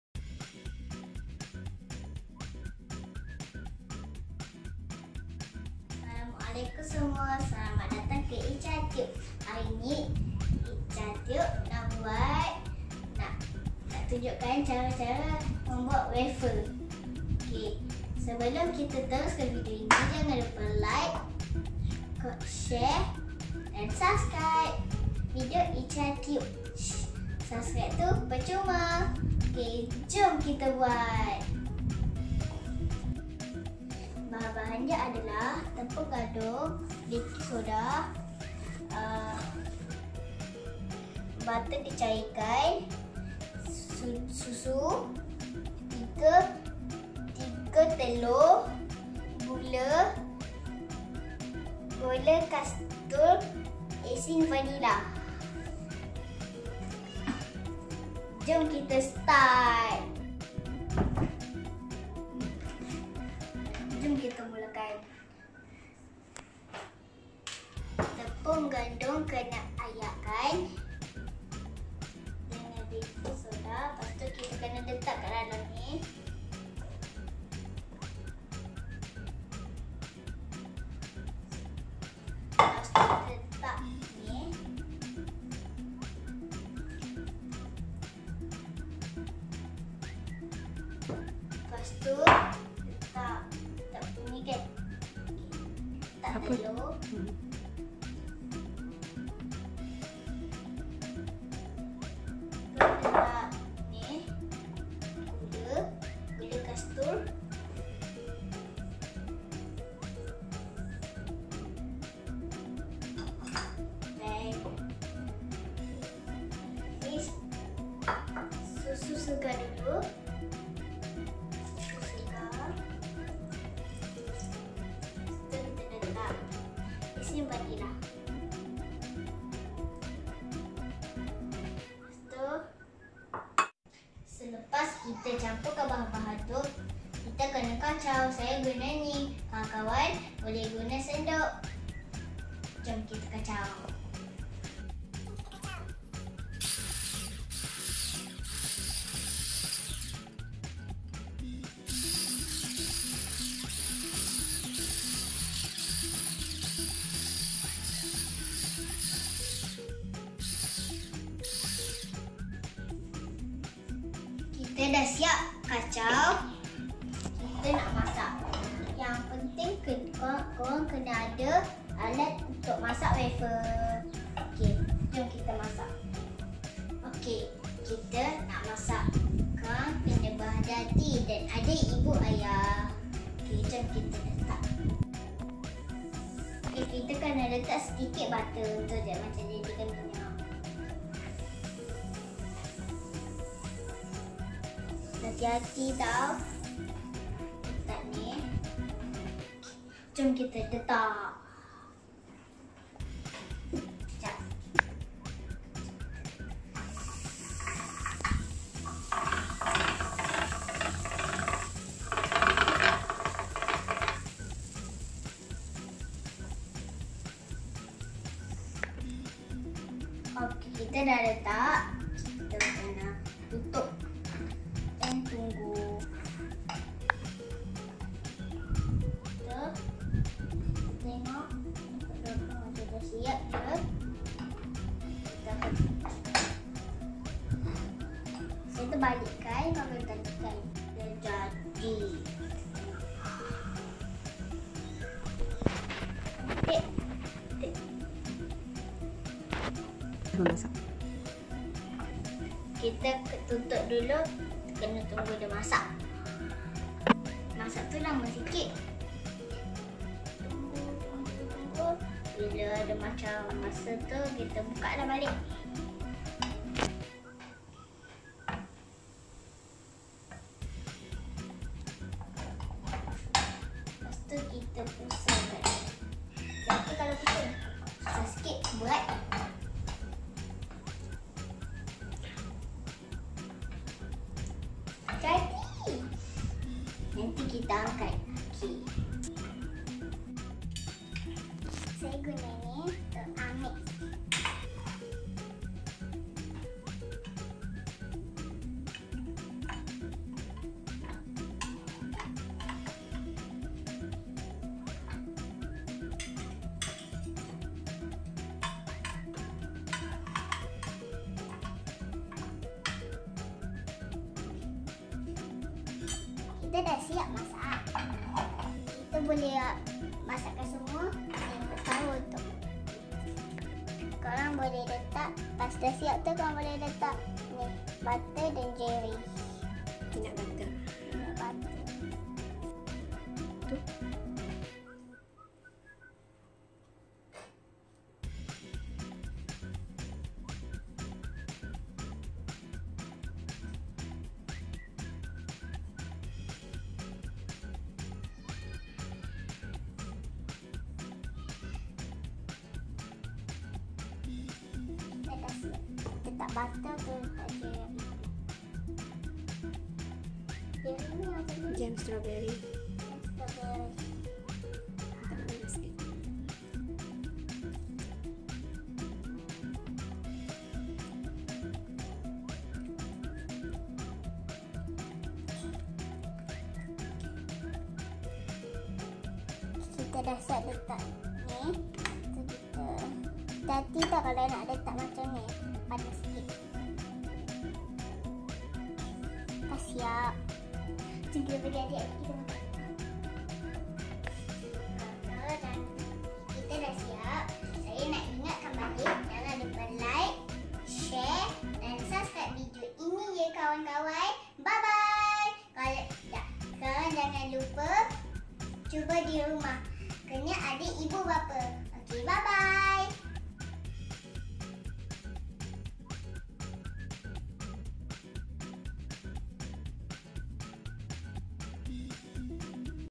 Jom masak WAFFLE bersama Echa
Echa Tube akan menunjukkan kawan-kawan cara untuk membuat waffle. Sangat mudah dan kawan-kawan boleh membuatnya sendiri di rumah. Paling penting jangan lupa utama kan keselamatan di dapur dan lakukan dengan pantauan ibu atau ayah ya. Jom kita start 😍❤️